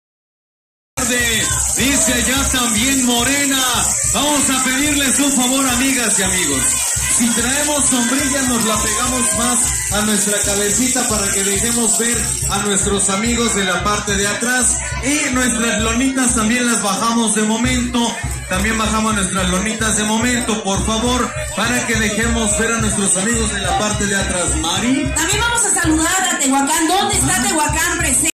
Lalo Castillo, candidato diputado federal por el distrito 14, llega al zócalo de Izúcar de Matamoros
Lalo Castillo, candidato a diputado federal por el distrito con cabecera en Izúcar de Matamoros arriba al zócalo en donde será el arranque de campaña del candidato a gobernador de Morena, Alejandro Armenta.